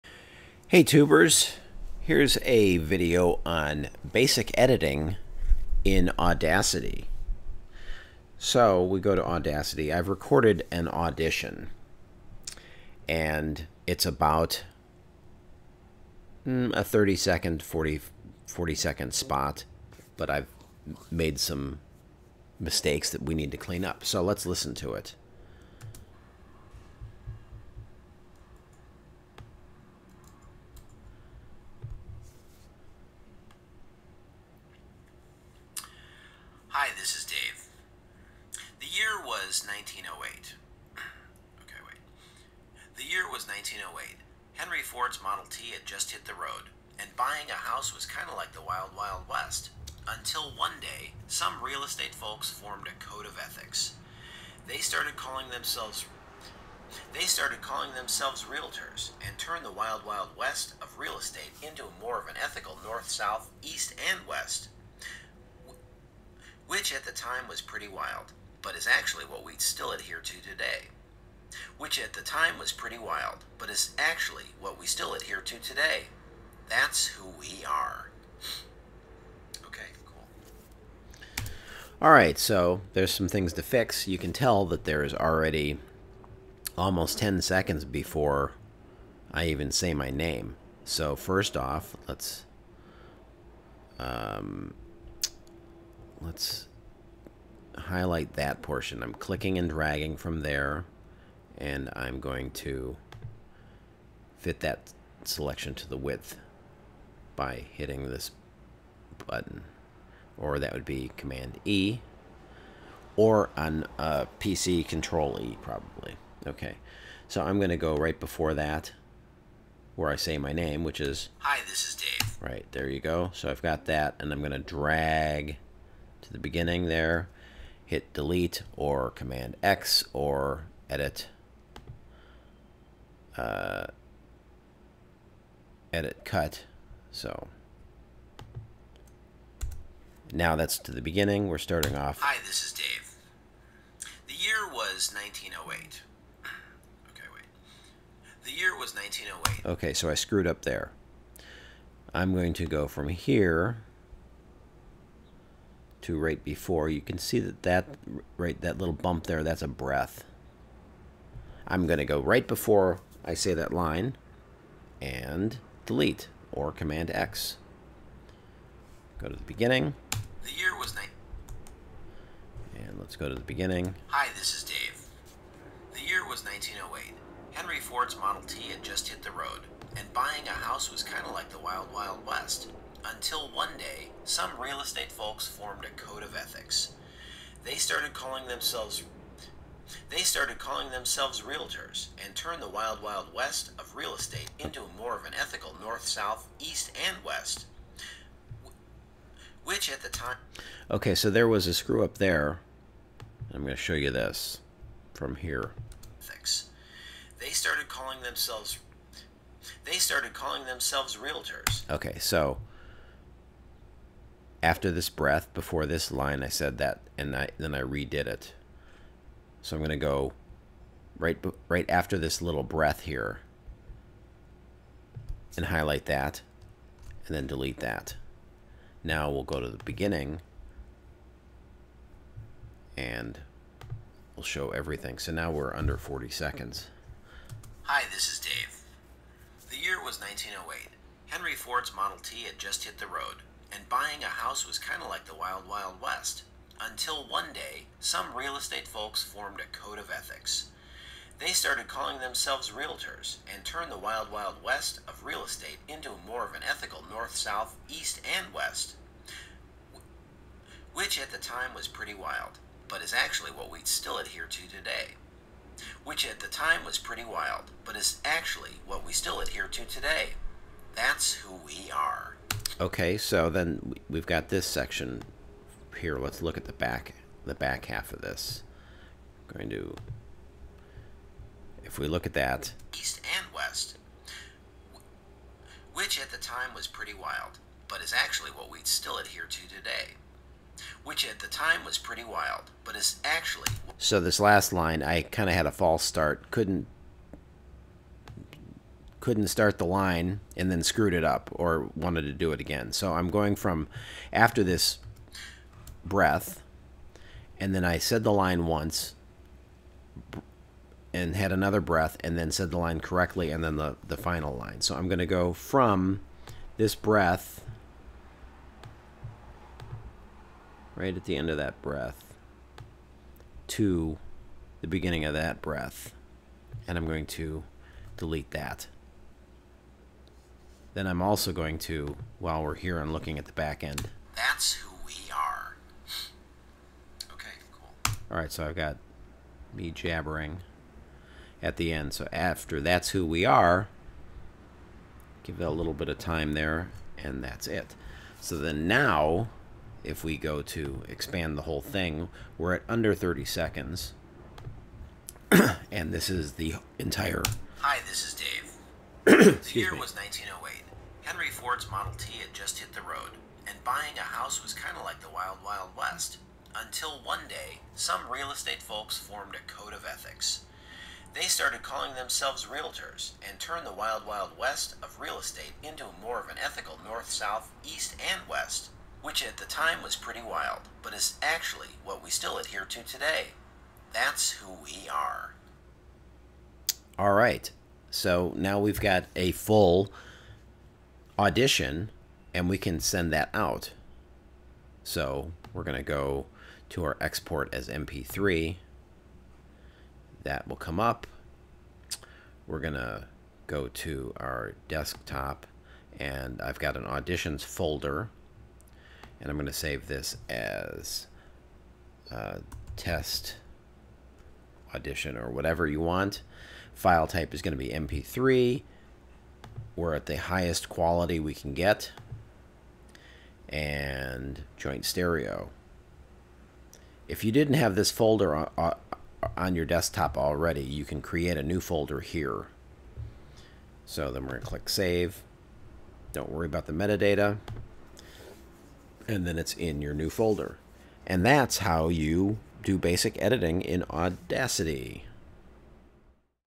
Basic editing for your voiceover audition in Audacity
This is a tutorial on editing a recorded audition and exporting it as an mp3 to your auditions folder.  I show how you can identify mistakes, false starts and breaths to have a cleaner audition.